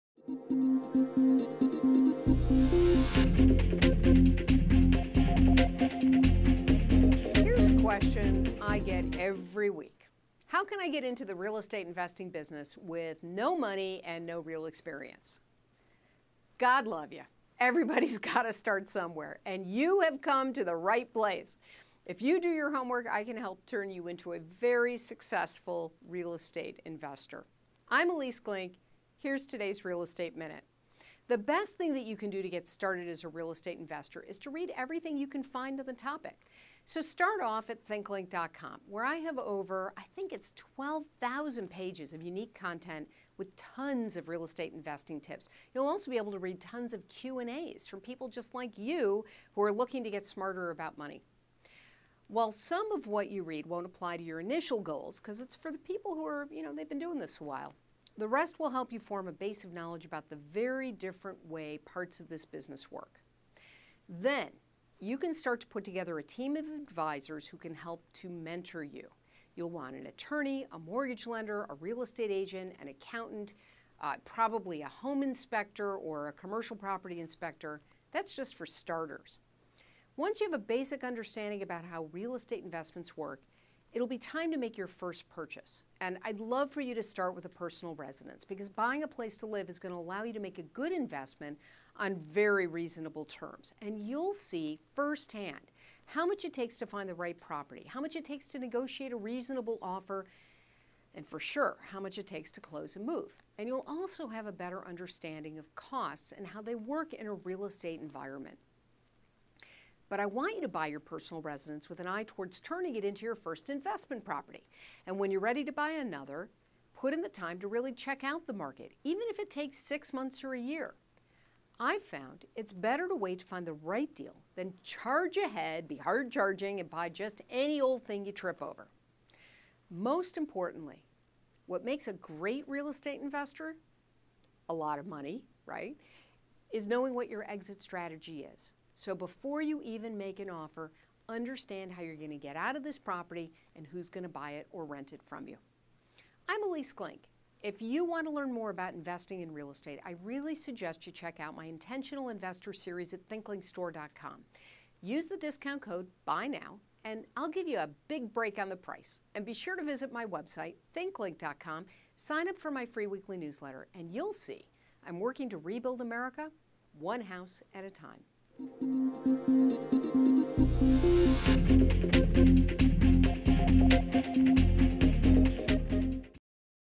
How to Become a Successful Real Estate Investor
Want to know how to make money by investing in real estate? I’ve got some advice that can work for anyone who wants to be a successful real estate investor.

Even if you’re starting out without much money or knowledge about the housing market, don’t be discouraged. You can learn to be a smart real estate investor before you even choose your first property or apply for a loan. Whether your goal is buying and renting or fixing and flipping, you can earn a living if you do your homework, find helpful mentors and lenders, and start with the basics. Buying real estate isn’t something to do without careful planning. This Real Estate Minute video will give you the basics, but check out the links below for more information that will help you learn to buy property for profit.